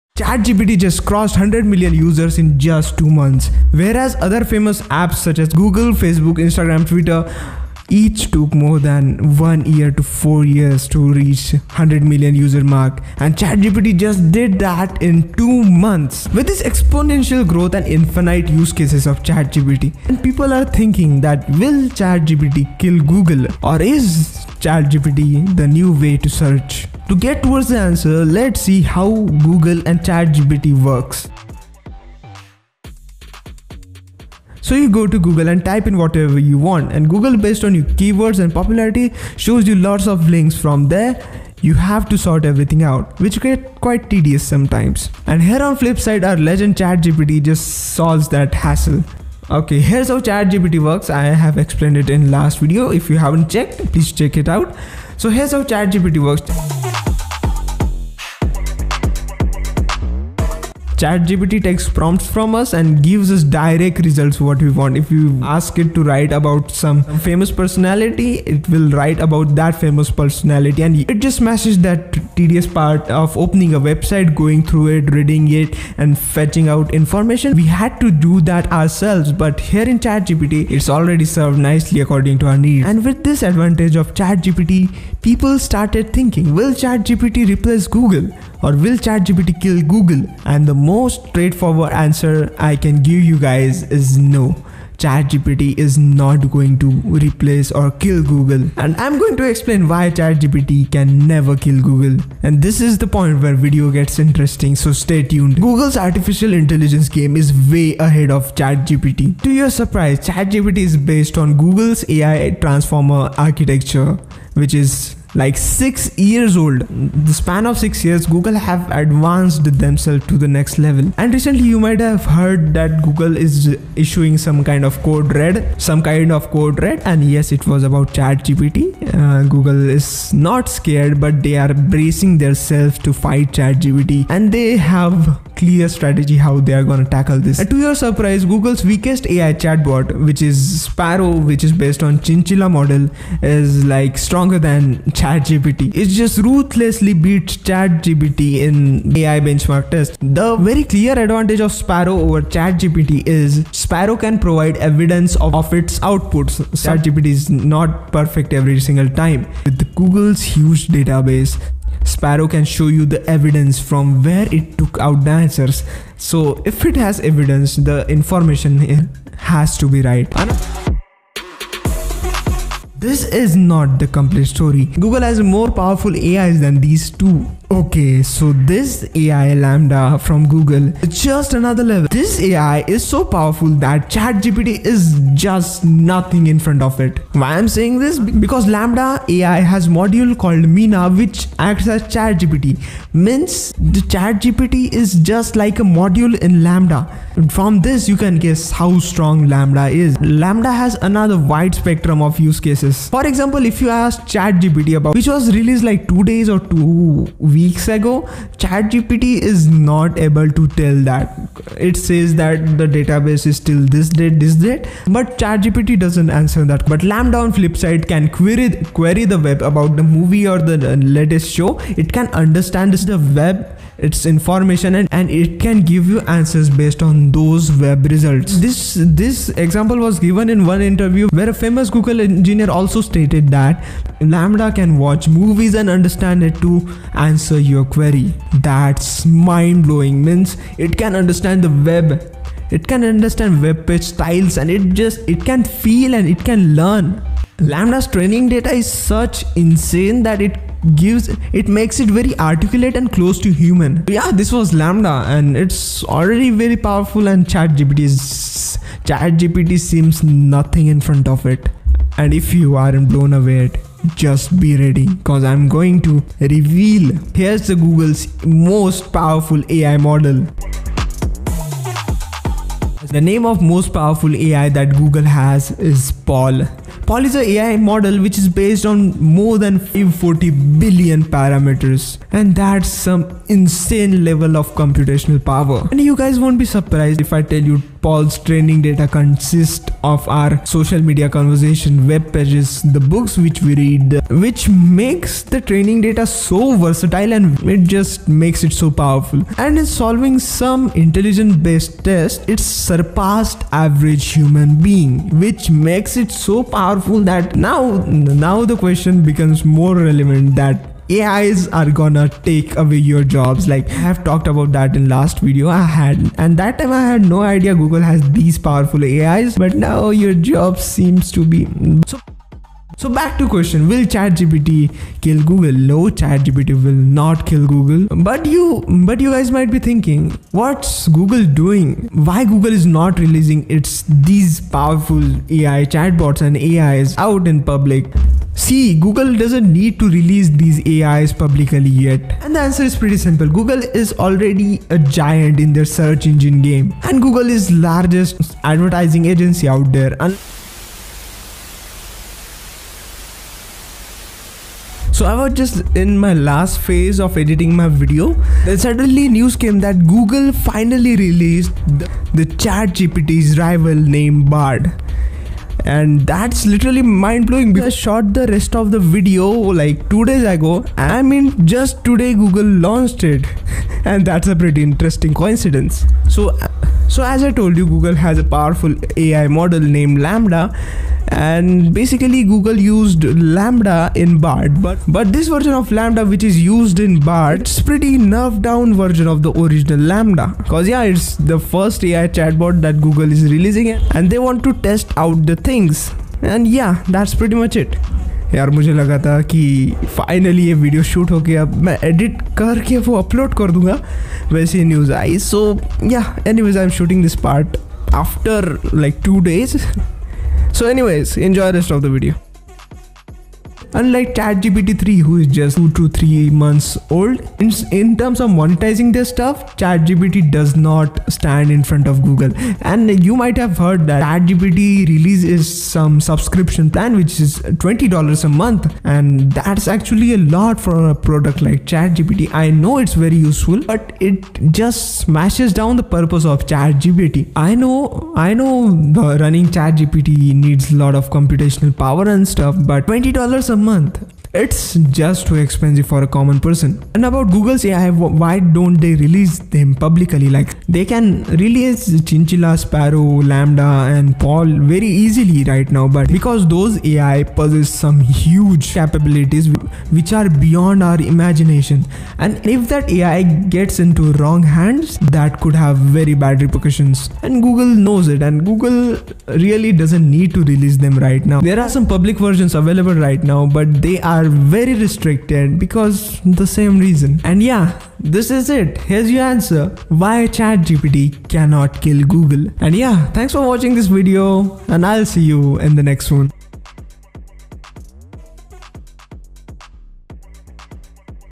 Chat GPT is about to DESTROY Google | Bard vs chat GPT
This Video Talks about how Chat GPT can naver kill or replace google
here I have also Explaned How different AI models from google works .

I have talked about LaMDA Sparrow PaLM thease are some of powerfull Ai Models that google uses
I have explained where Chat GPT lacks and where google has advantage over Chat GPT